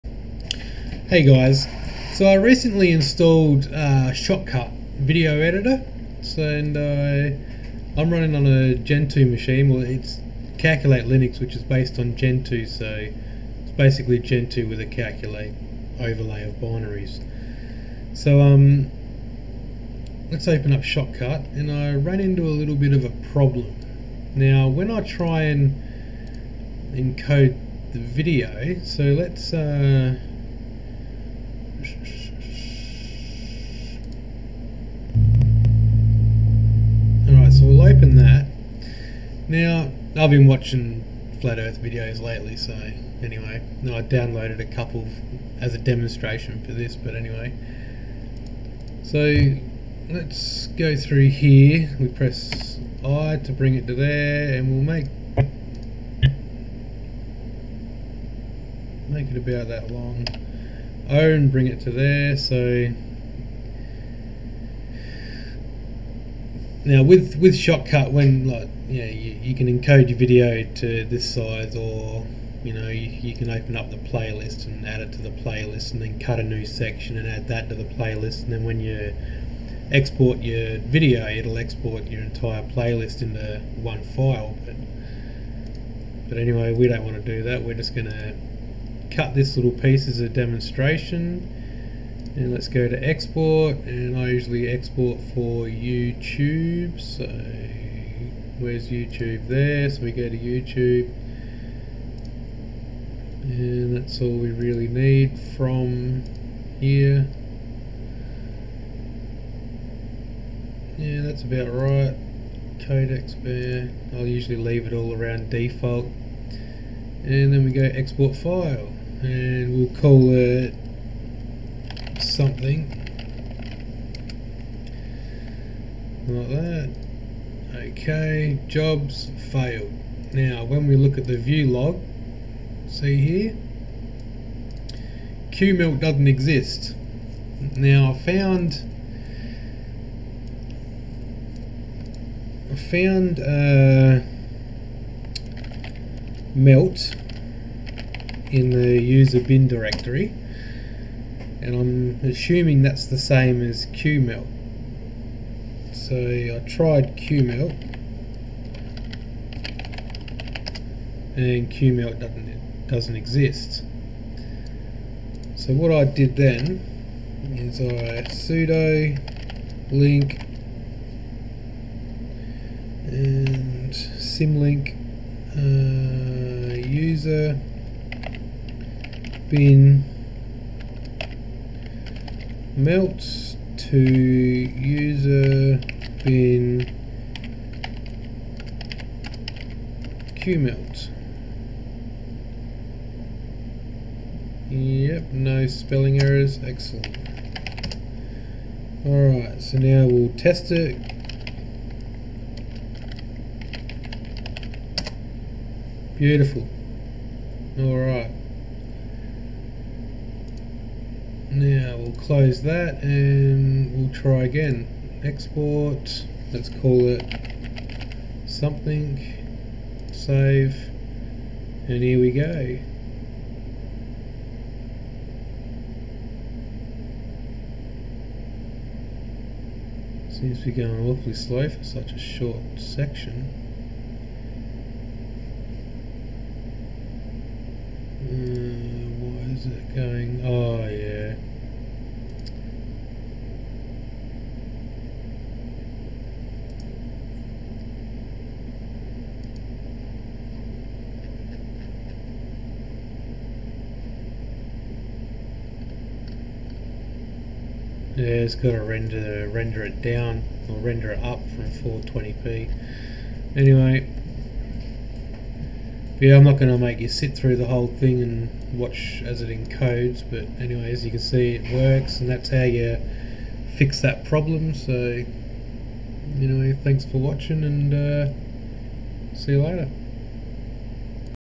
How to fix ShotCut export failure Calculate Linux (Gentoo)
This will show you how to fix the /usr/bin/qmelt not found error while trying to render your video in ShotCut.